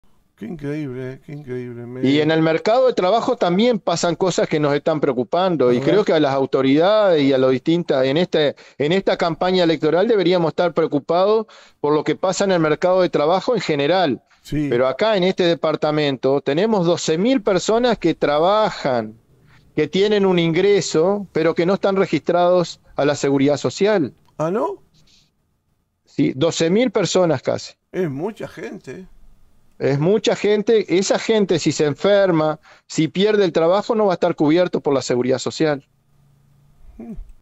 Ramón Ruiz, Representante de los Trabajadores en el BPS. Trabajadores en negro en Colonia.
Somos un medio de comunicación con más de 150 años en la región oeste del departamento de Colonia.